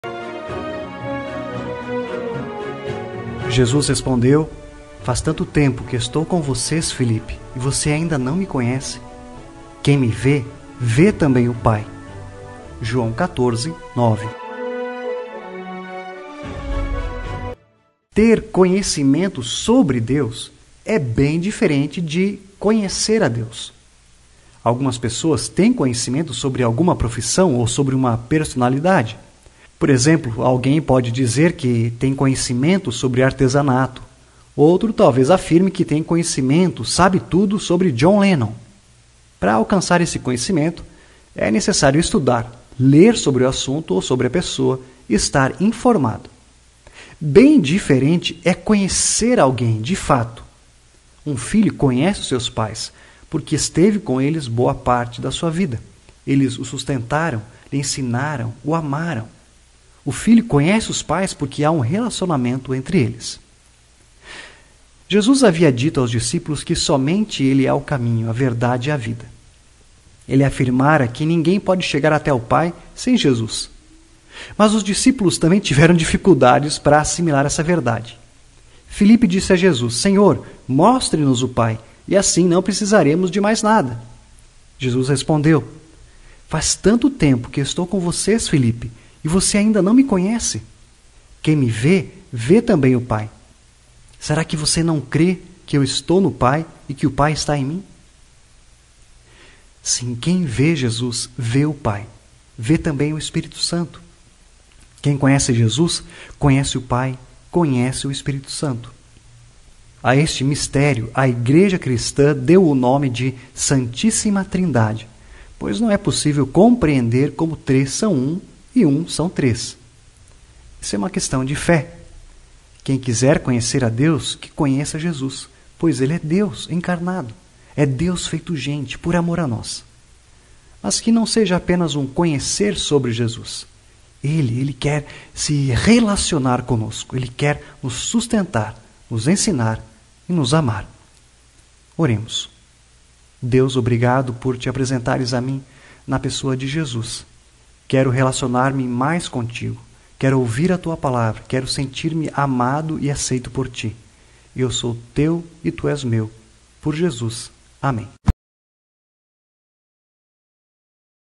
Conhecer a Deus: segredos revelados em João 14.9 | Diário De Deus
Envie para seus Contatos!!!    Faça seu pedido de oração! Tenha um dia abençoado!
Conhecer a Deus: segredos revelados em João 14.9
 Em João 14.9, há um segredo revelado para aqueles que desejam conhecer a Deus em toda a sua plenitude. Neste vídeo, desvendamos o significado por trás dessa passagem bíblica e compartilhamos insights valiosos que podem transformar a forma como você enxerga e se relaciona com o Divino.